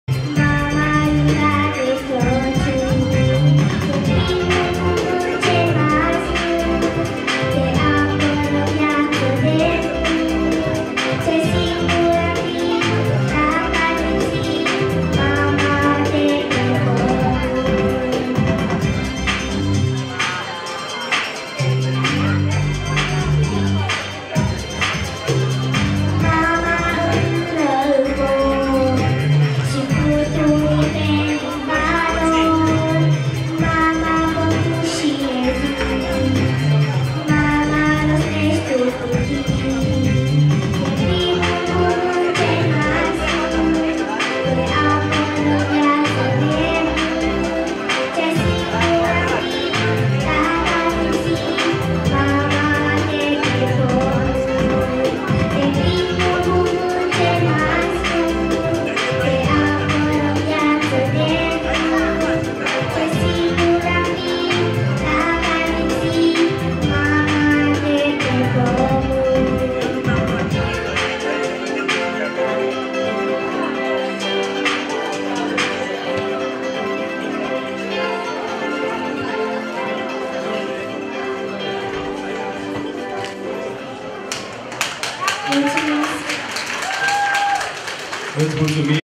Martisor 2014
Eveniment organizat de Colegiul National de Arte Dinu Lipatti.